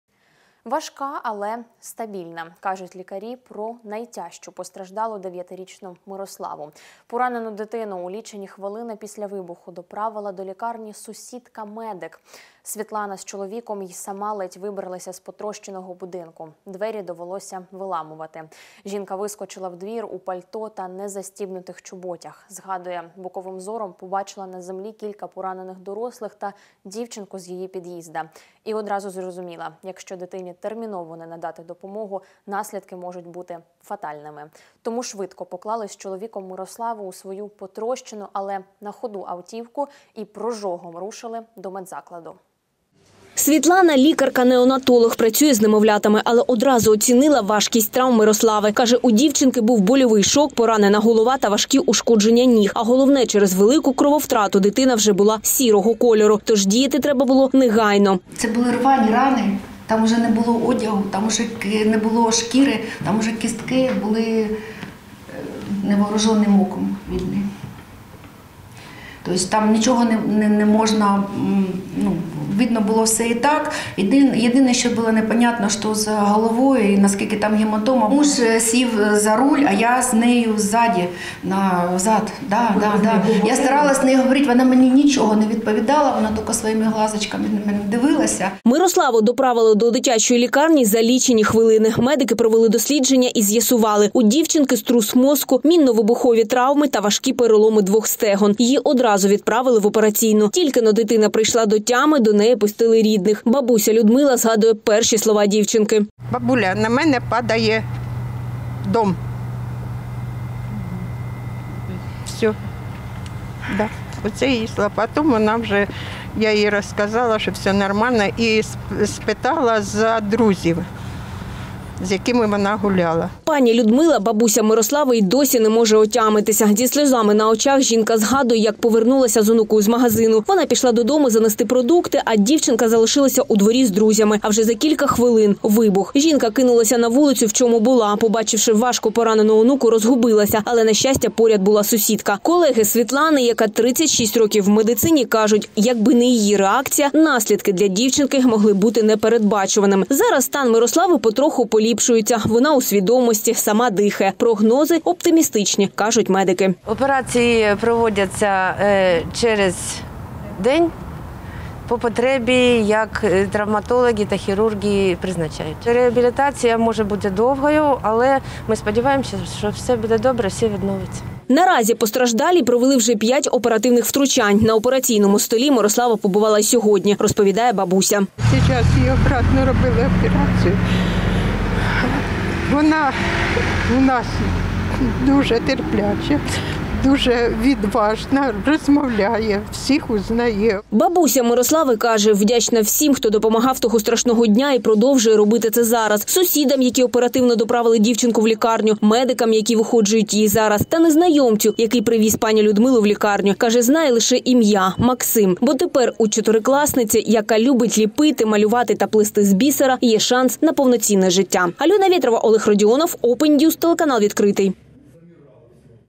“Бабуля на мене дом падає”: це перші слова постраждалої під час ракетного удару по Дніпру дівчинки
Стан дівчинки важкий, але стабільний. Кажуть лікарі про найтяжчу постраждалу під час ракетного удару рф по Дніпру 14 січня.  9-річна Мирослава поступово оговтується. Хоча реабілітація буде тривалою. Зазначимо, поранену дитину у лічені хвилини після вибуху доправила до лікарні сусідка - медик. Світлана з чоловіком й сама ледь вибралися з потрощеного будинку, двері довелося виламувати. Жінка вискочила в двір у пальто, незастібнутих чоботях та дипломатом з документами. Каже, боковим зором побачила на землі кілька поранених дорослих та дівчинку з її підїзда. І одразу зрозуміла, якщо дитині терміново не надати допомогу, наслідки можуть бути фатальними. Тому швидко поклали з чоловіком Мирославу у свою потрощену, але на ходу автівку, і прожогом рушили до медзакладу.